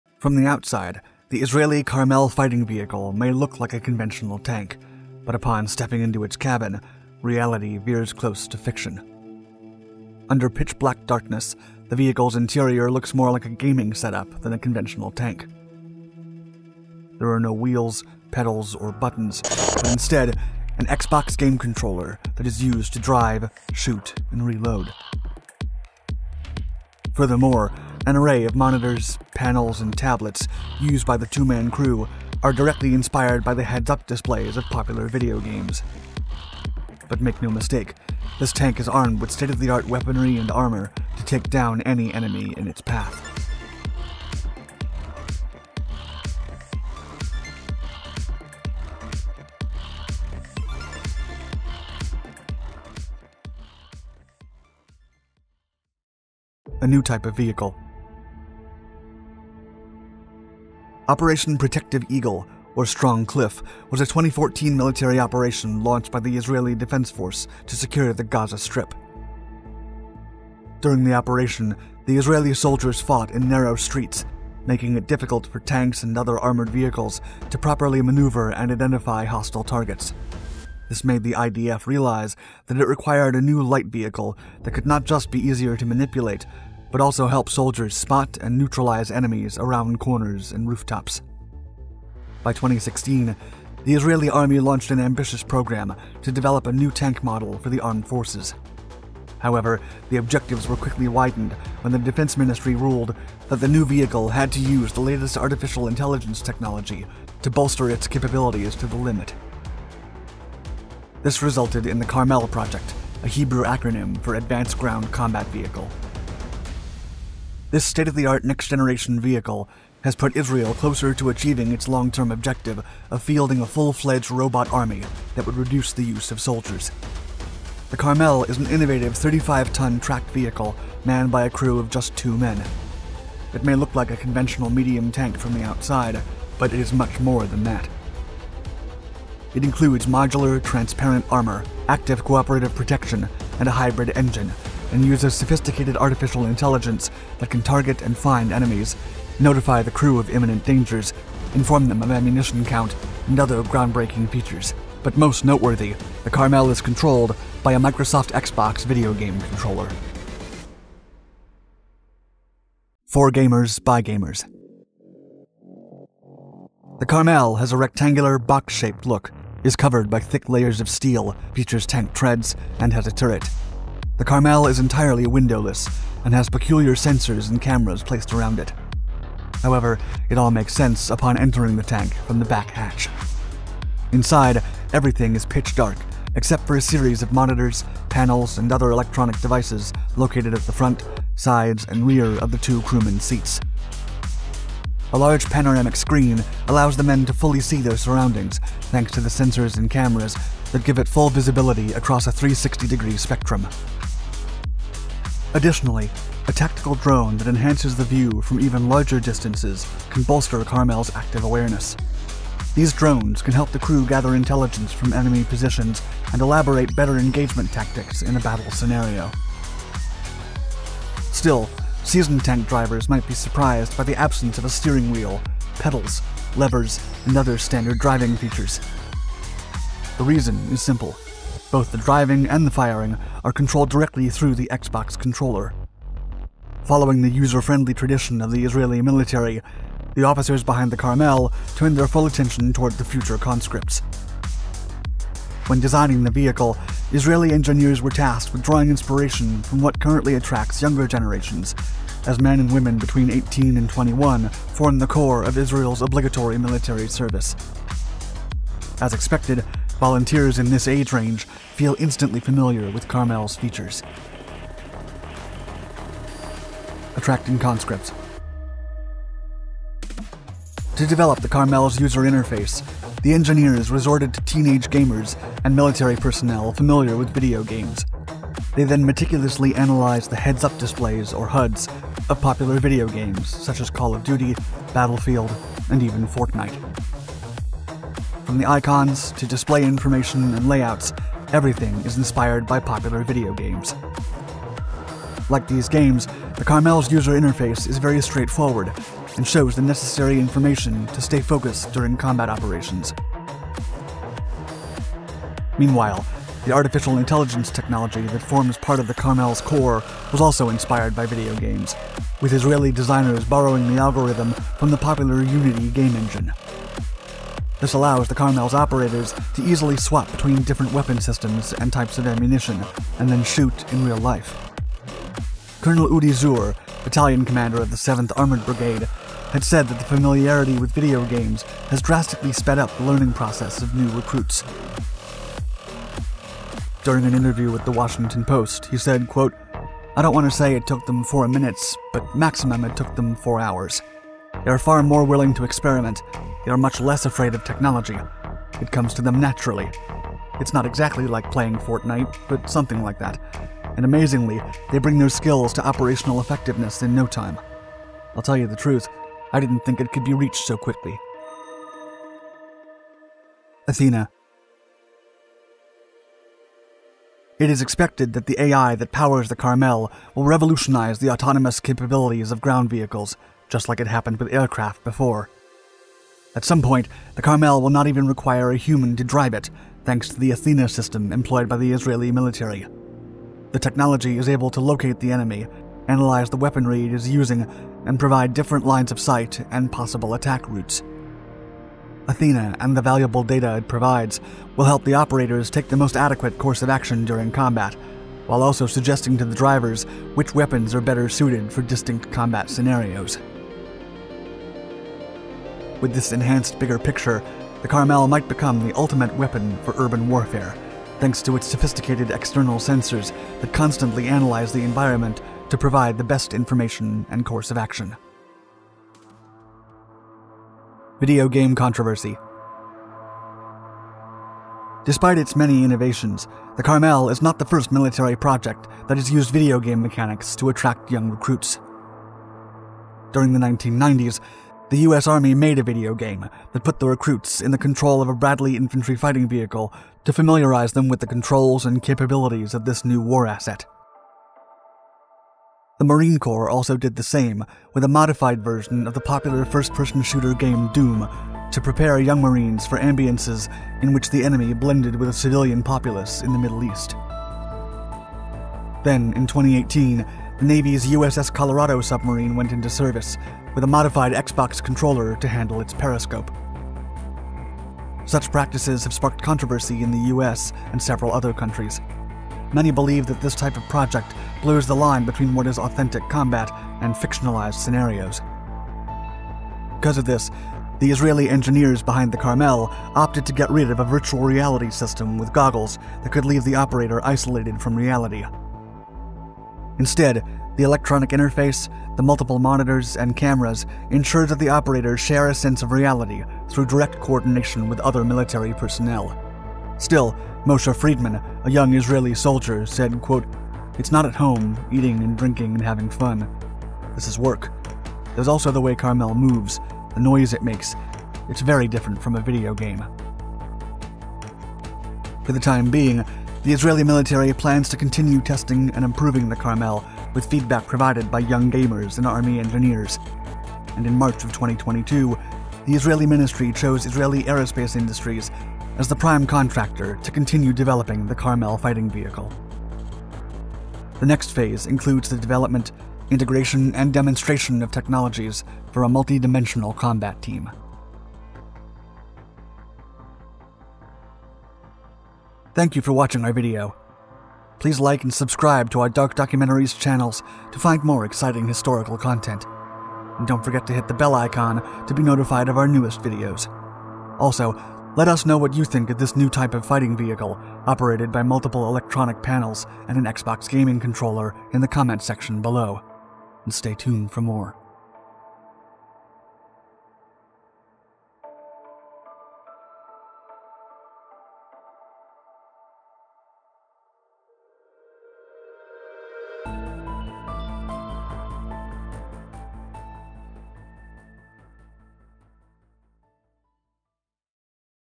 Project Carmel - Future Fighting Vehicle
From the outside, the Israeli Carmel Fighting Vehicle may look like a conventional tank, but upon stepping into its cabin, reality veers close to fiction.

Under pitch-black darkness, the vehicle’s interior looks more like a gaming setup than a conventional tank. There are no wheels, pedals, or buttons, but instead, an Xbox video game controller that is used to drive, shoot and reload.

Furthermore, an array of monitors, panels, and tablets used by the two-man crew are directly inspired by the Heads Up Displays of popular video games.

But make no mistake, this tank is armed with state-of-the-art weaponry and armor to take down any enemy on its path…